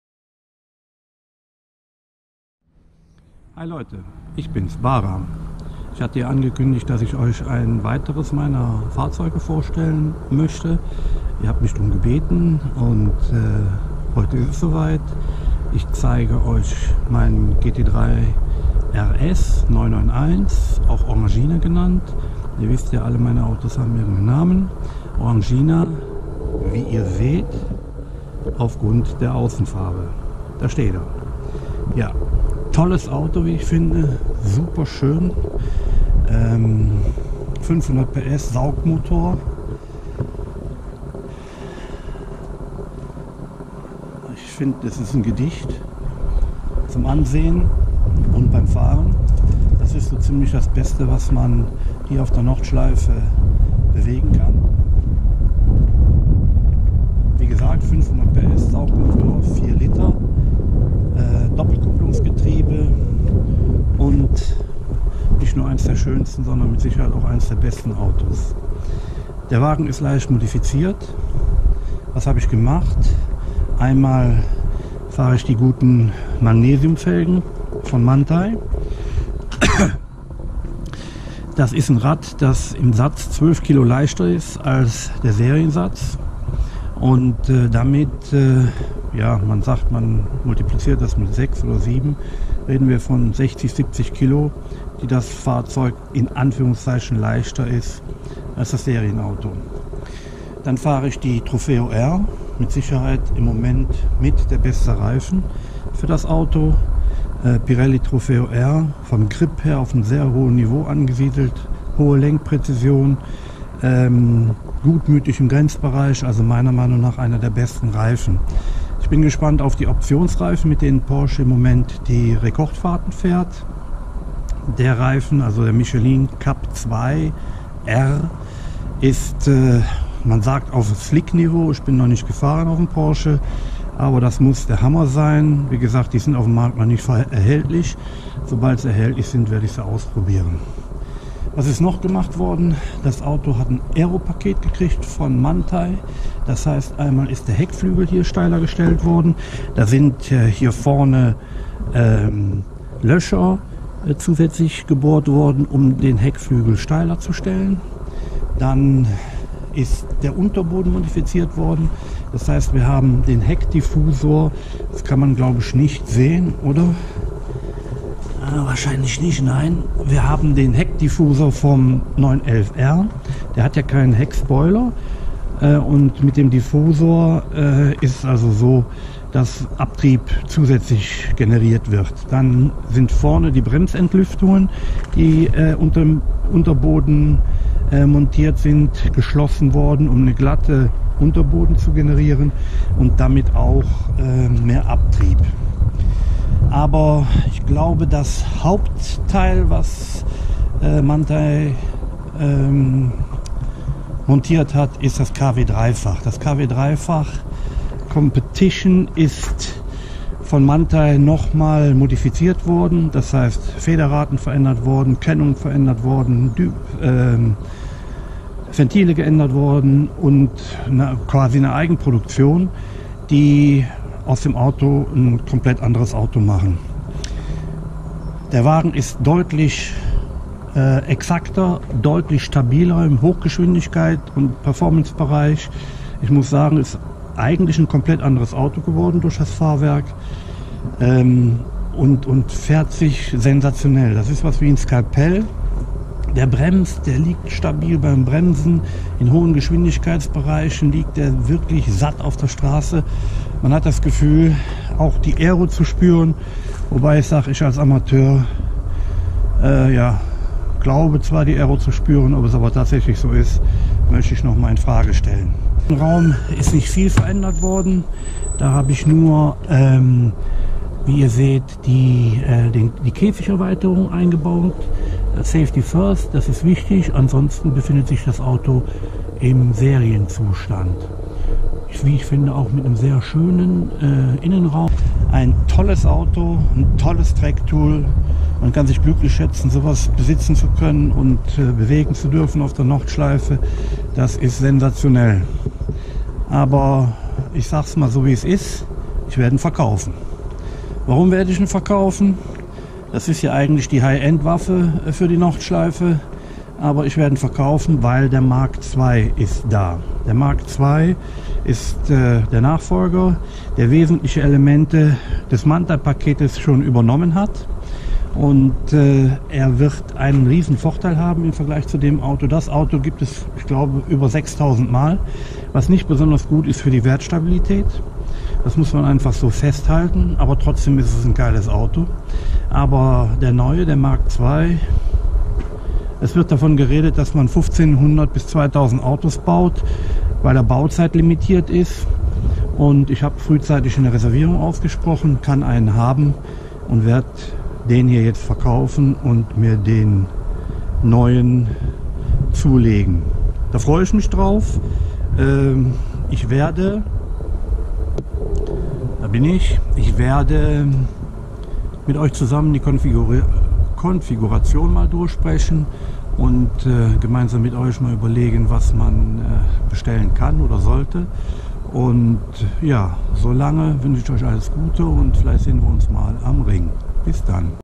walk around GT3 RS "Orangina"
Nürburgring Nordschleife – Bridge to Gantry
Porsche 991 GT 3 RS, 500 HP
Suspension: Raeder/Manthey KW 3 fach Competition
Manthey Aerokit
Pirelli Trofeo R
Cam GoPro Hero 5
Driver Bahram